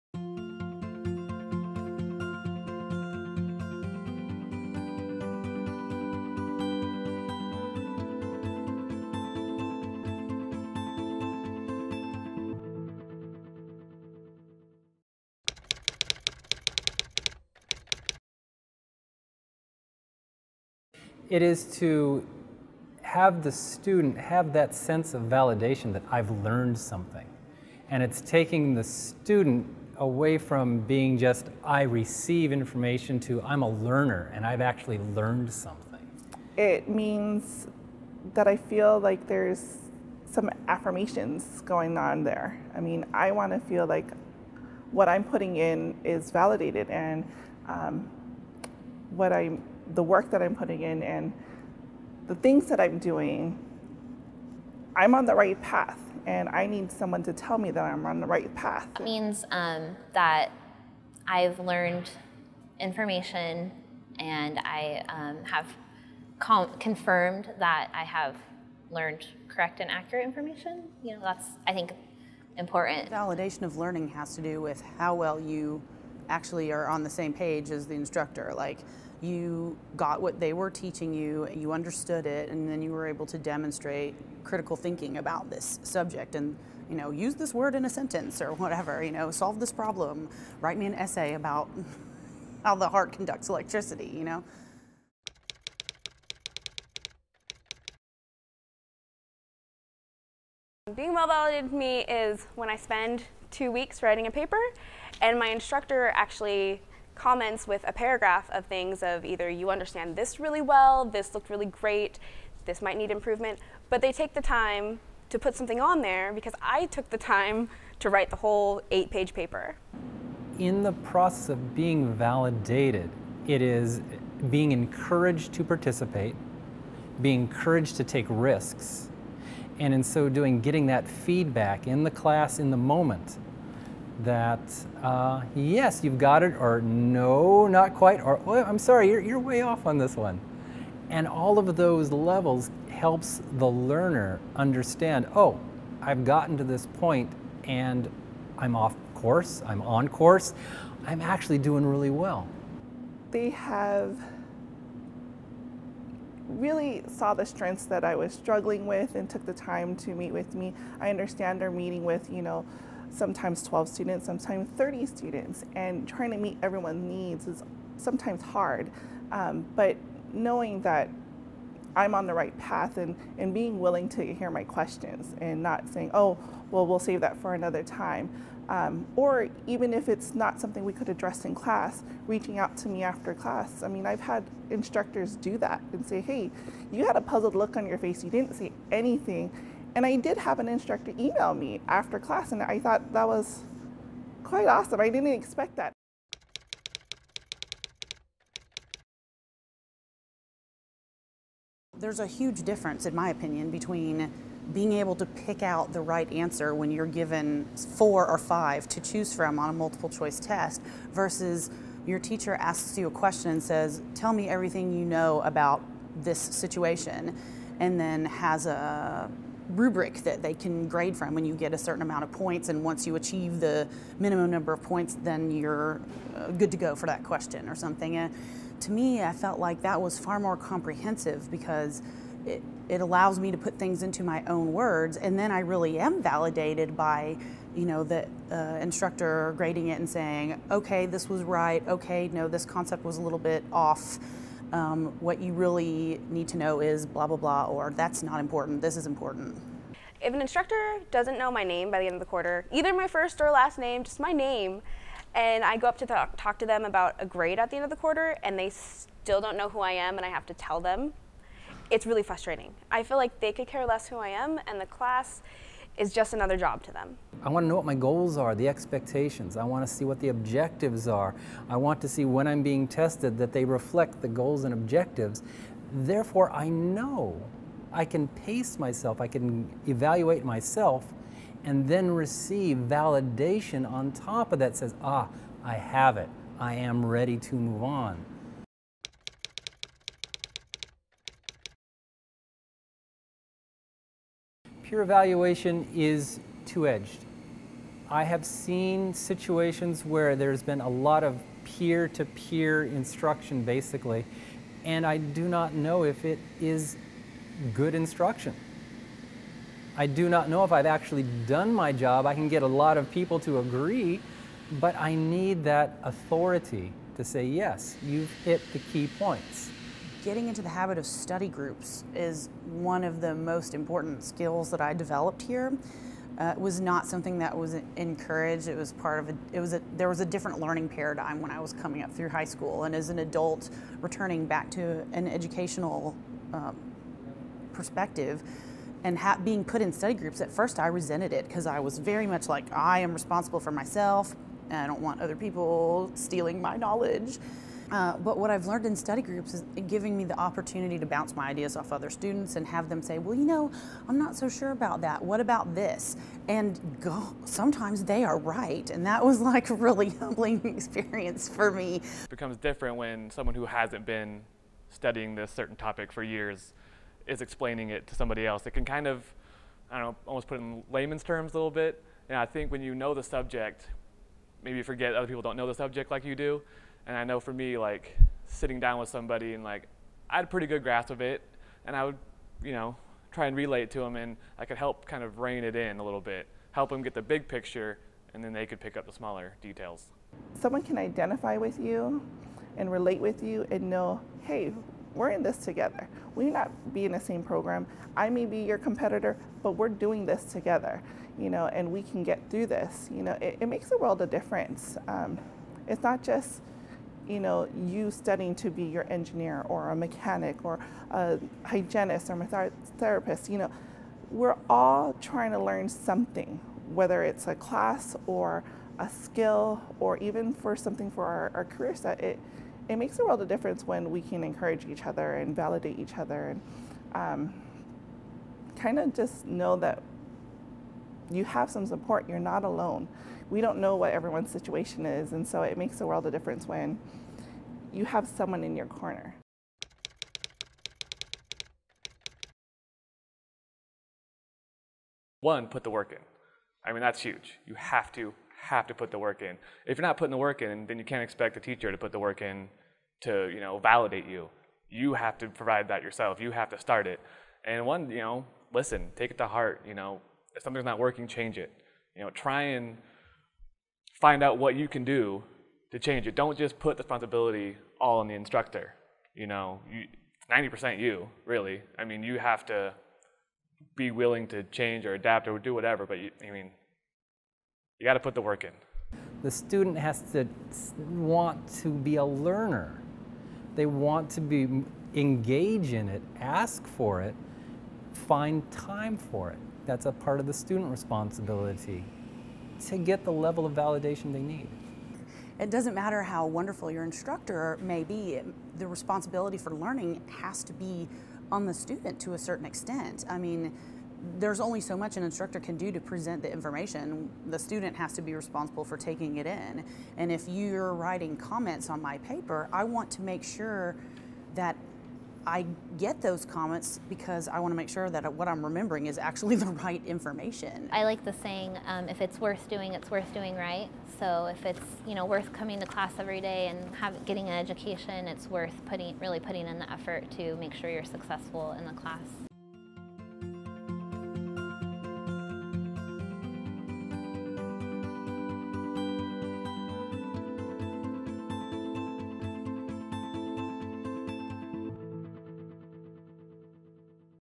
LWTech Validation In Teaching
What Validation in Teaching means to the students at LWTech.